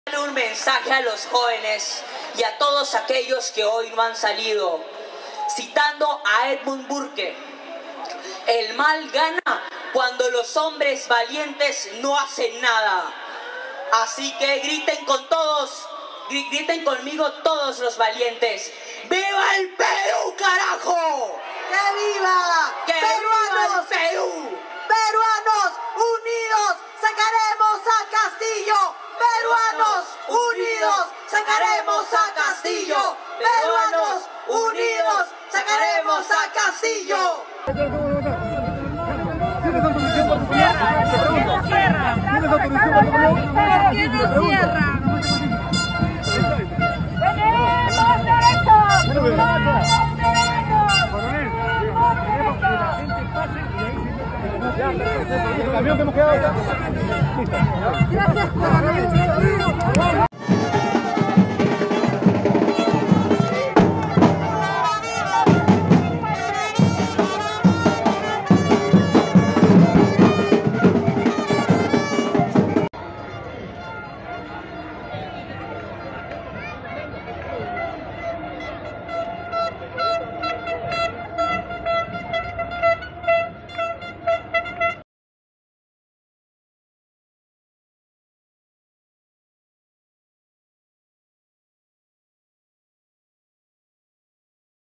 Polémica en marcha a favor de la vacancia de Pedro Castillo
Nacional ¿Qué opinas? La marcha a favor de la vacancia de Pedro Castillo generó polémica al evidenciarse escenas como la del empresario Erasmo Wong, quien utilizó sus influencias llamando a altos mandos de la policía para romper el cordón que impedía el paso de los manifestantes. La presencia del grupo extremista la Resistencia liderando la marcha y un menor de 14 años pronunciando un discurso en el que pide a los jóvenes a salir a luchar por la salida del presidente, también llamaron la atención.